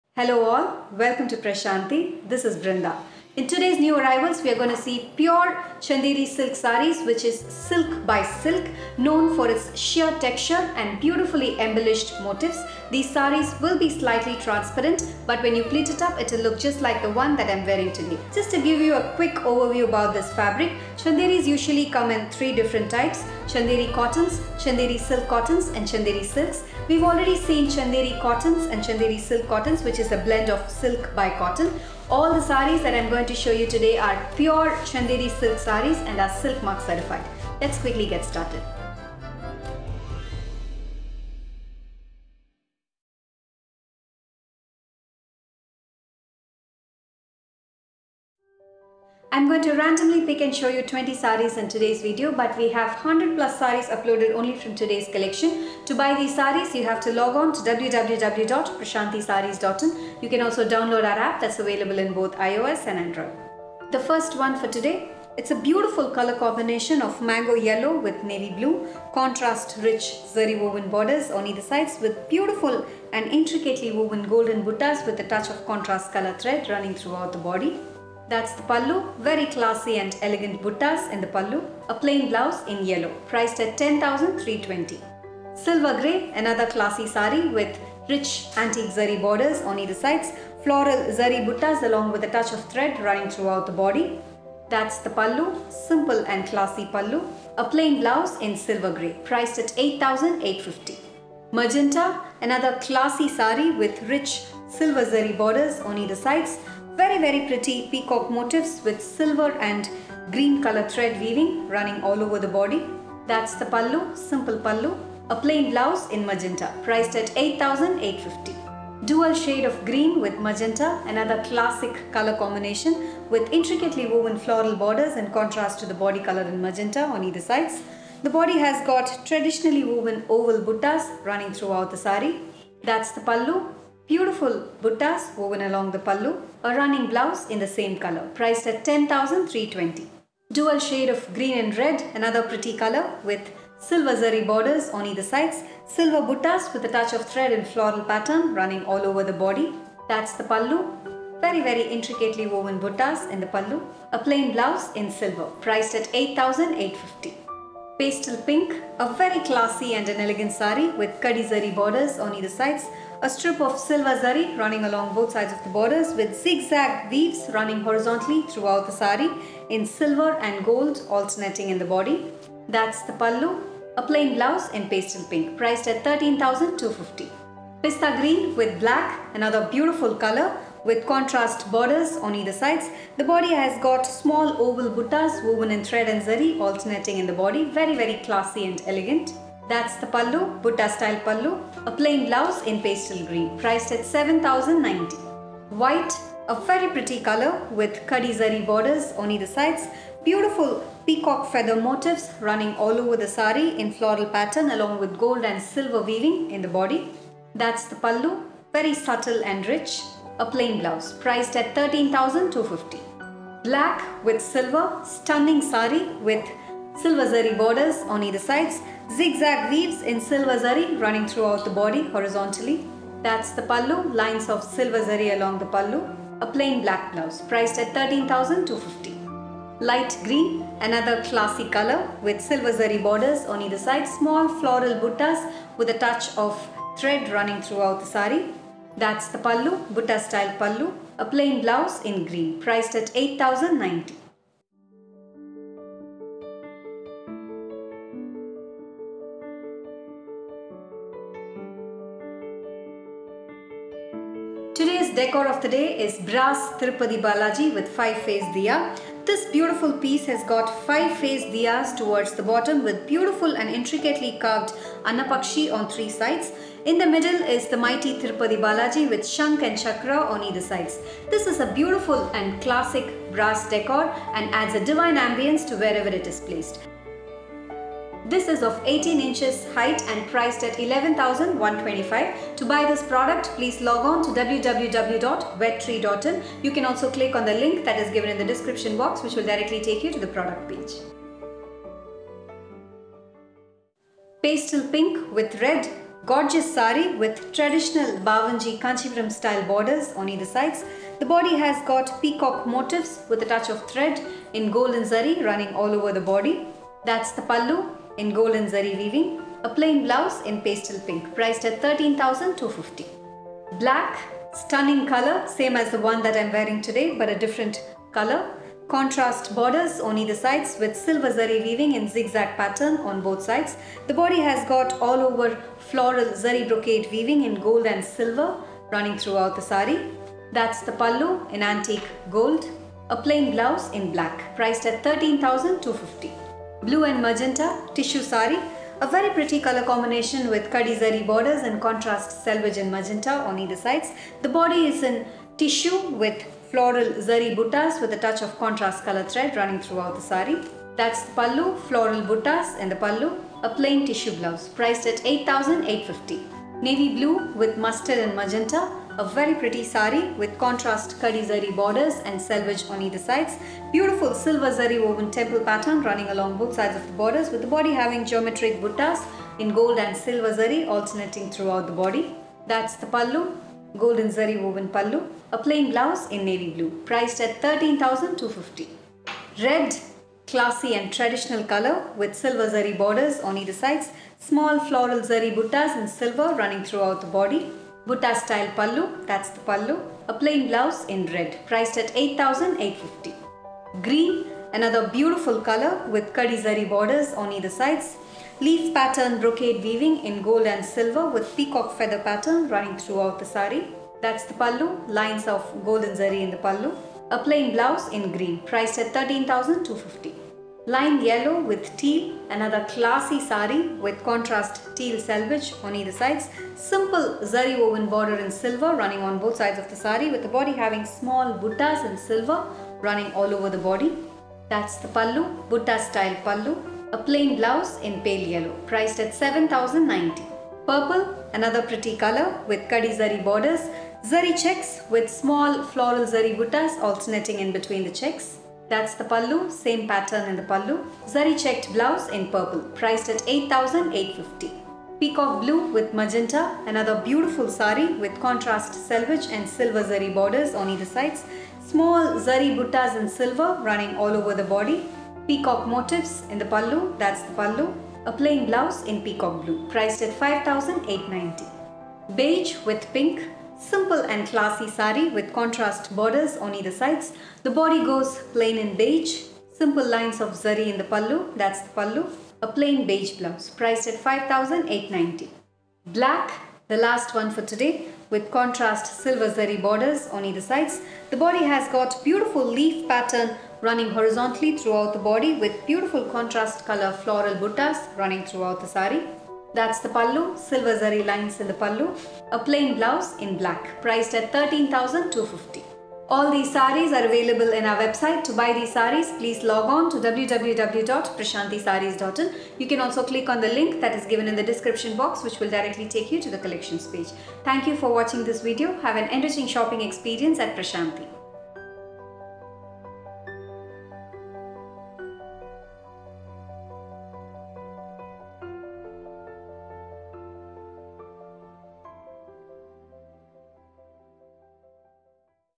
Pure Chanderi Silks by Prashanti | 16 November 2021
Decor of the day - Wedtree's Brass Balaji Shunk Chakra Diya available @

Once preferred by the queens of India, the Chanderi sarees are a class apart among handwoven garments. The production of Chanderi fabrics is a family business that has been running for a few centuries. This fabric is exclusive to Chanderi, a town in Madhya Pradesh. The versatility of Chanderi textile makes it a favourite canvas for fashion connoisseurs to practise their art on.

Chanderi silk sarees are woven Pure Silk by Silk. The refined weaves are very well suited for festivals, Poojas and traditional occasions. They have a wild assortment of buttis, zaris, intricate patterns etc done on the saree palette. Discover new designs and colours of Chanderi silk sarees at Prashanti.